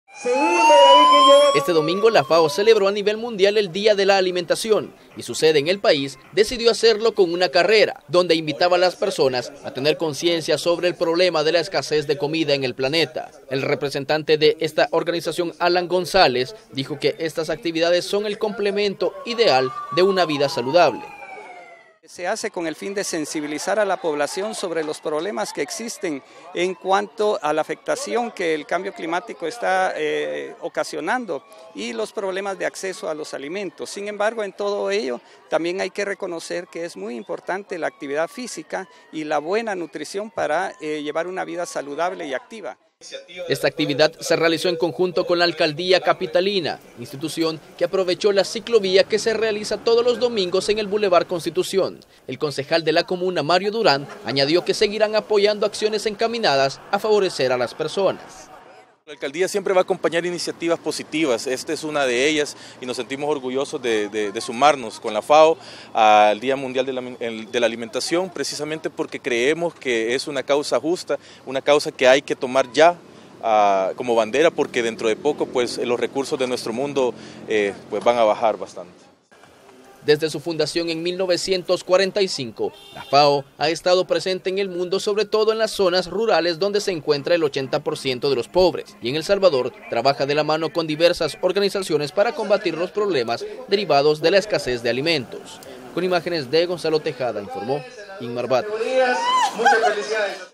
CN: Carrera FAO
La FAO y la alcaldía de San Salvador, conmemoraron el Día Mundial de la Alimentación con una carrera cuyo objetivo fue concienciar a la población sobre el problema de la escasez de comida en el planeta. Veamos.